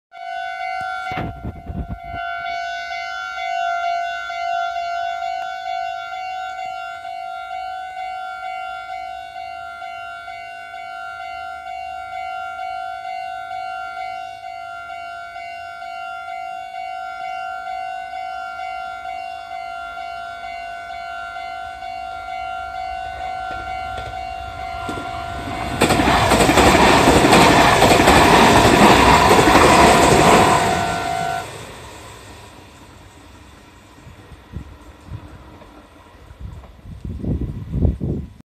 名古屋鉄道3150系＋3300系
名古屋鉄道3150系＋3300系を撮影してみました